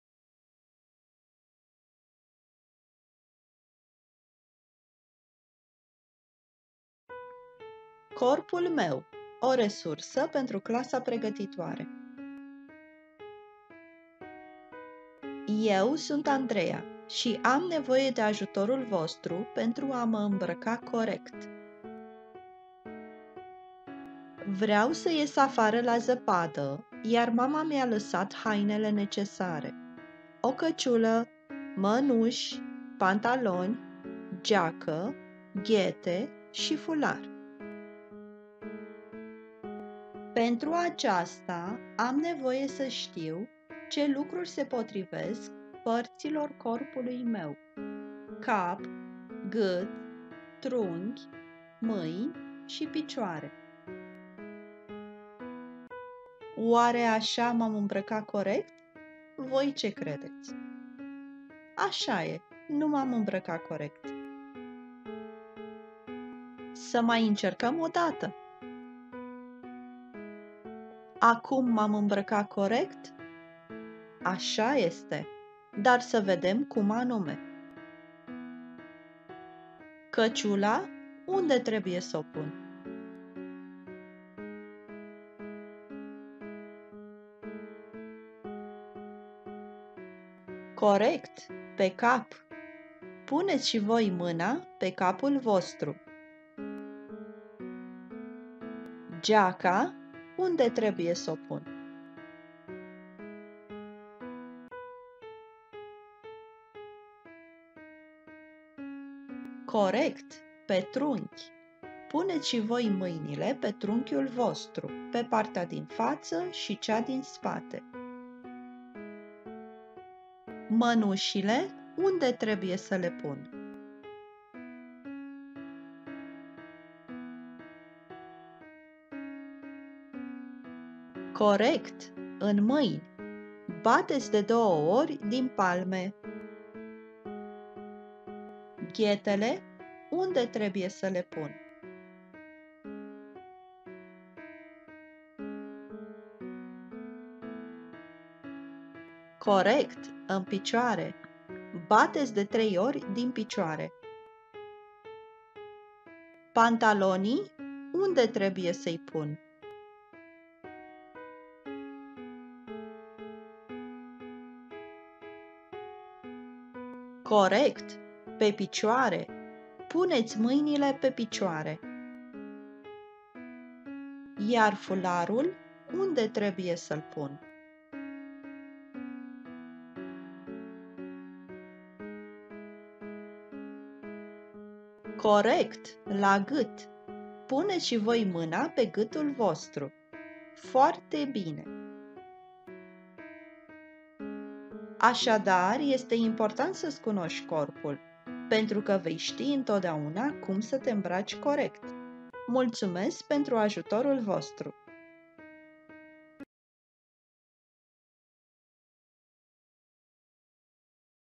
Corpul meu
Resursa descrie schema corporală prin raportarea la o activitate practică – cea de a te îmbrăca corect prin asocierea fiecărui articol de îmbrăcăminte cu partea corpului pentru care este utilizat. Jocul de asociere a articolelor de îmbrăcăminte cu partea corespunzătoare a corpului pornește de la o variantă greșită, solicitând elevilor să ofere soluția corectă și să le indice pe propriul corp.

Autor:  Ioana Georgiana Soare